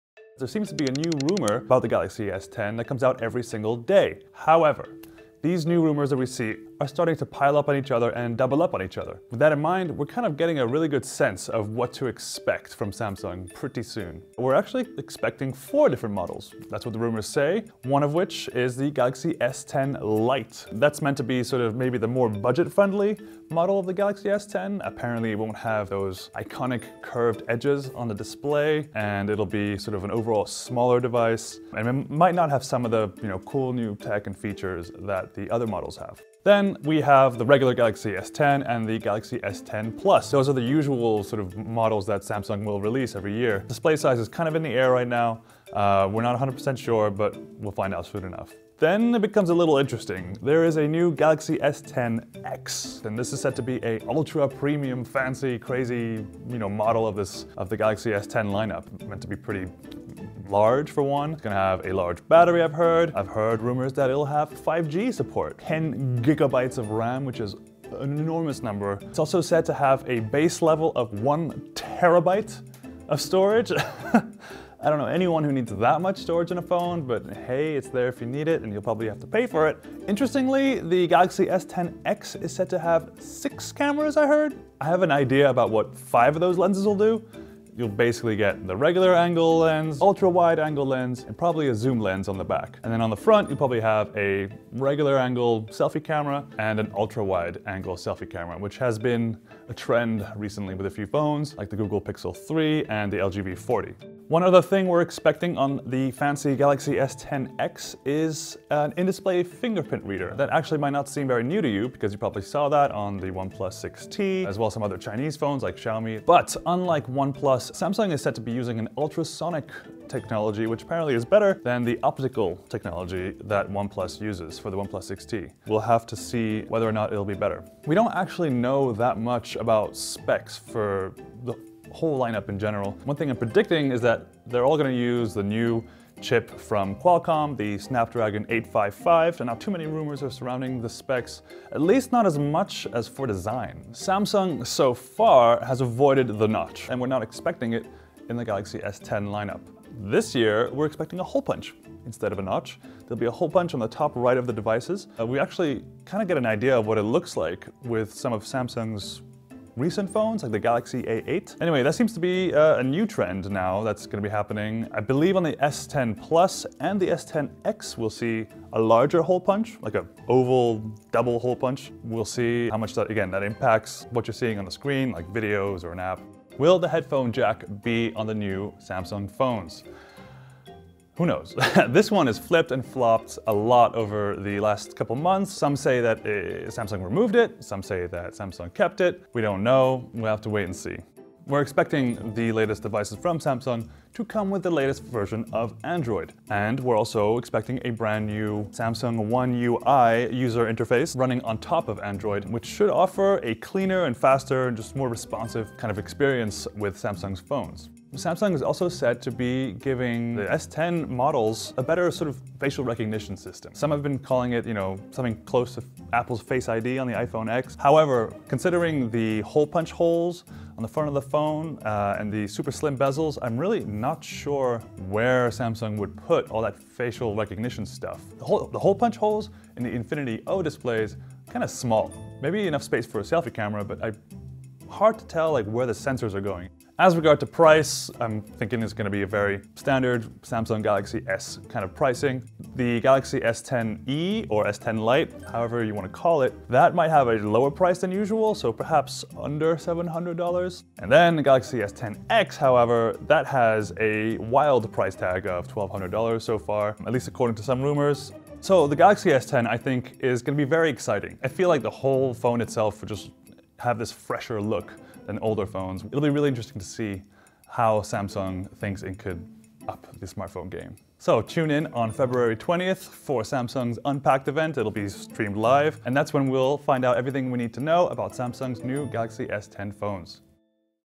Samsung Galaxy S10 Rumors — Everything We Know
Samsung's Galaxy S10 phones have leaked several times before launch. The Samsung's Galaxy S10 could have an in-display fingerprint reader, up to six cameras, up to 10GB of RAM, and a hole-punch display. The Galaxy S10 is shaping up to be Samsung's best phone yet. Business Insider's Antonio Villas-Boas breaks down all of the rumors surrounding Samsung's Galaxy S10 phones.

Following is a transcript of the video:

There seems to be a new rumor about the Galaxy S10 that comes out every single day. However, these new rumors that we see are starting to pile up on each other and double up on each other. With that in mind, we're kind of getting a really good sense of what to expect from Samsung pretty soon.

We're actually expecting four different models. That's what the rumors say. One of which is the Galaxy S10 Lite. That's meant to be sort of maybe the more budget-friendly model of the Galaxy S10. Apparently it won't have those iconic curved edges on the display. And it'll be sort of an overall smaller device. And it might not have some of the, you know, cool new tech and features that the other models have. Then we have the regular Galaxy S10 and the Galaxy S10+. Those are the usual sort of models that Samsung will release every year. Display size is kind of in the air right now. We're not 100% sure but we'll find out soon enough.

Then it becomes a little interesting. There is a new Galaxy S10 X. And this is said to be an ultra-premium, fancy, crazy, you know, model of this, of the Galaxy S10 lineup. It's meant to be pretty large for one. It's gonna have a large battery I've heard. I've heard rumors that it will have 5G support. 10 gigabytes of RAM, which is an enormous number. It's also said to have a base level of one terabyte of storage. I don't know anyone who needs that much storage in a phone. But hey, it's there if you need it. And you'll probably have to pay for it.

Interestingly, the Galaxy S10 X is said to have six cameras I heard. I have an idea about what five of those lenses will do. You'll basically get the regular-angle lens. Ultra-wide angle lens. And probably a zoom lens on the back. And then on the front, you'll probably have a regular-angle selfie camera. And an ultra-wide angle selfie camera, which has been a trend recently with a few phones like the Google Pixel 3 and the LG V40. One other thing we're expecting on the fancy Galaxy S10 X is an in-display fingerprint reader. That actually might not seem very new to you because you probably saw that on the OnePlus 6T, as well as some other Chinese phones like Xiaomi. But, unlike OnePlus, Samsung is said to be using an ultrasonic technology, which apparently is better than the optical technology that OnePlus uses for the OnePlus 6T. We'll have to see whether or not it'll be better.

We don't actually know that much about specs for the whole lineup in general. One thing I'm predicting is that they're all gonna use the new chip from Qualcomm. The Snapdragon 855. So not too many rumors are surrounding the specs. At least not as much as for design.

Samsung so far has avoided the notch. This year we're expecting a hole-punch instead of a notch. There'll be a hole-punch in the top right of the devices. We actually kind of get an idea of what it looks like with some of Samsung's recent phones. Like the Galaxy A8. Anyway, that seems to be a new trend now that's gonna be happening. I believe on the S10+ and the S10 X we'll see a larger hole-punch. Like an oval, double hole-punch. We'll see how much, that again, that impacts what you're seeing on the screen like videos or an app.

Will the headphone jack be on the new Samsung phones? Who knows. This one has flipped and flopped a lot over the last couple months. Some say that Samsung removed it. Some say that Samsung kept it. We're expecting the latest devices from Samsung to come with the latest version of Android. And we're also expecting a brand new Samsung One UI, user interface, running on top of Android, which should offer a cleaner and faster and just more responsive kind of experience with Samsung's phones. Samsung is also said to be giving the S10 models a better, sort of, facial recognition system. Some have been calling it, you know, something close to Apple's FaceID on the iPhone X. However, considering the hole-punch holes on the front of the phone and the super slim bezels, I'm really not sure where Samsung would put all that facial recognition stuff